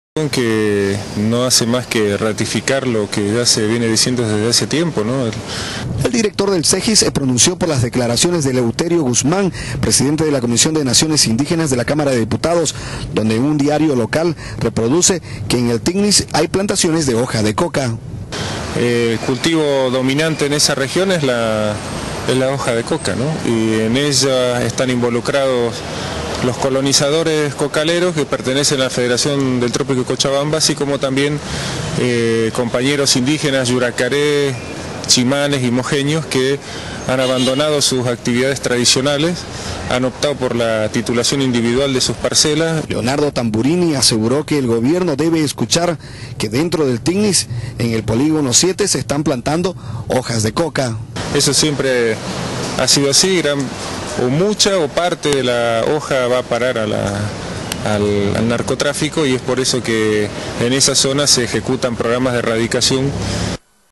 Coca ilegal en el Tipnis
Director del Cejis opina al respecto.